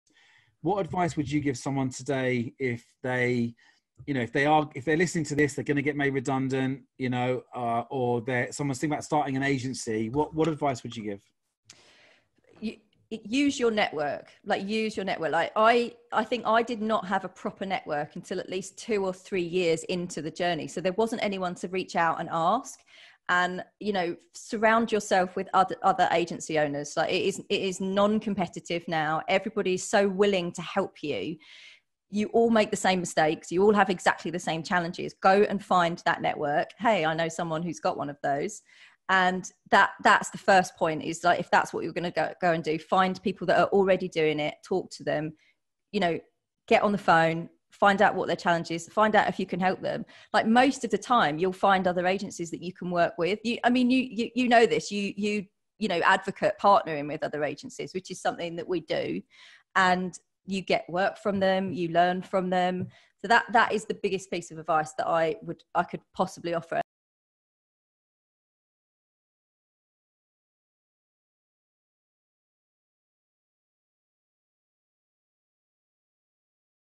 Agencyphonics Clip :Use your Network
This is from a recent vodcast with Kelly Molson where she talks about the use of having a network with other agencies.